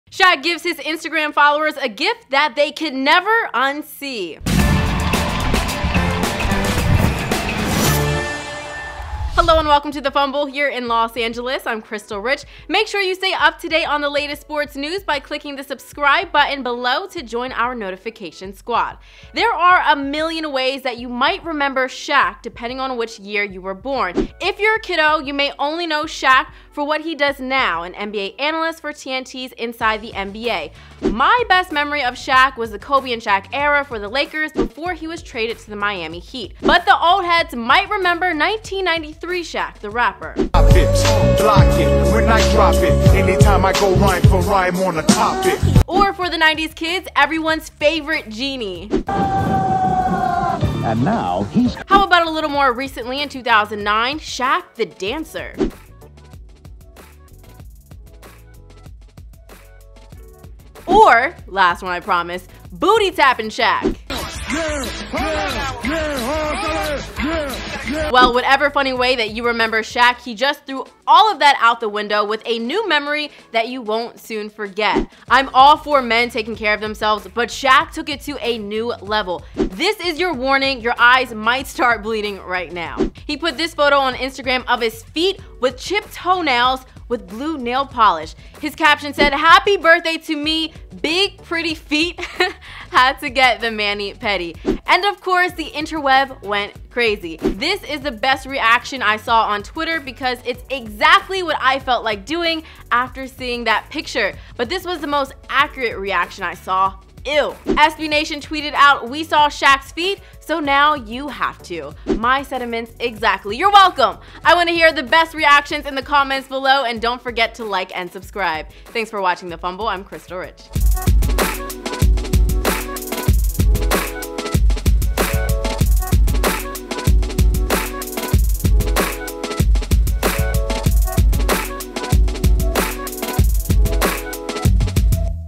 Shaq's Pedicured Feet Are the NASTIEST Sh!t You'll See All Day
There are a million ways that you might remember Shaq, depending on which year you were born. Whatever funny way you remember Shaq, he just threw ALL of that out the window with a new memory that you won’t soon forget, now matter how hard you try. Remember his nasty ass feet? They just got nastier with the help of blue toenail polish.